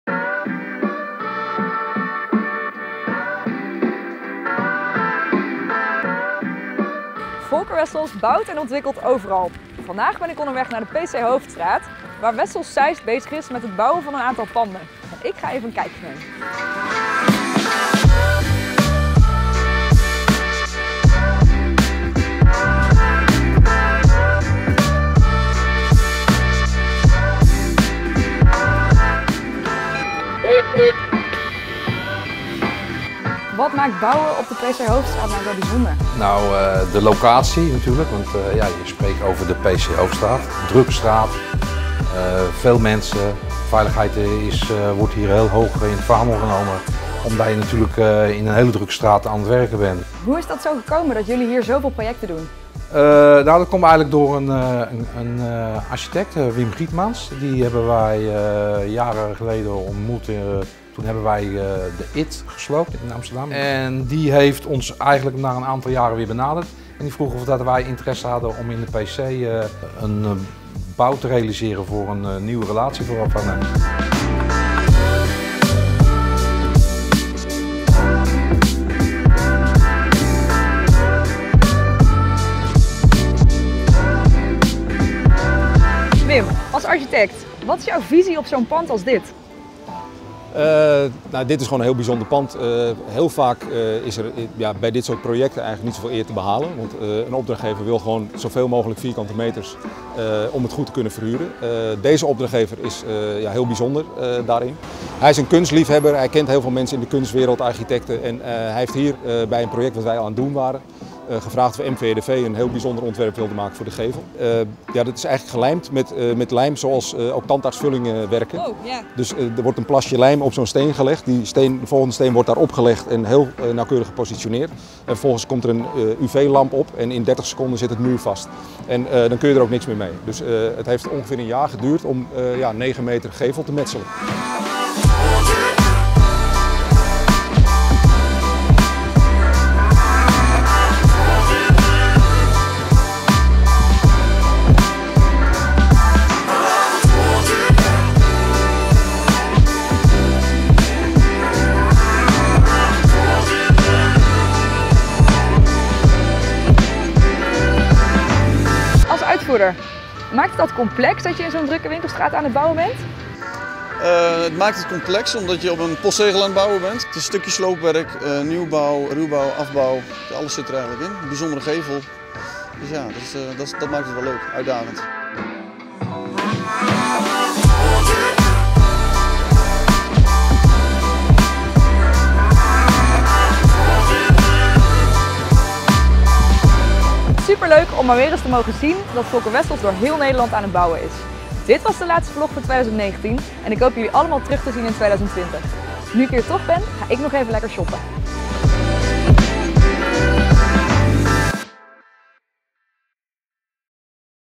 Ook dit is VolkerWessels vlog #12: PC Hooftstraat
VolkerWessels bouwt en ontwikkelt overal…zo ook op de PC Hooftstraat in Amsterdam. De meest exclusieve winkelstraat van Nederland is al jaren het werkterrein van VolkerWessels onderneming Wessels Zeist.

In de laatste vlog van 2019 spreekt VolkerWessels collega Amber met projectleider Robert van der Hoef en uitvoerder Richard van het Ende over deze bijzondere projectlocatie. Wat betekent het om te bouwen in druk binnenstedelijk gebied en wat maakt deze straat zo bijzonder?

Wessels Zeist werkt op de PC Hooftstraat nauw samen met architect Wim Gietermans, die o.a. de bouw van de beroemde kristallen gevel van het winkelpand van Hermes bouwkundig heeft begeleid. “We werken hier voor een opdrachtgever die niet alleen maar zoveel mogelijk verhuurbare vierkante meters wil creëren, maar écht iets bijzonders wil neerzetten.”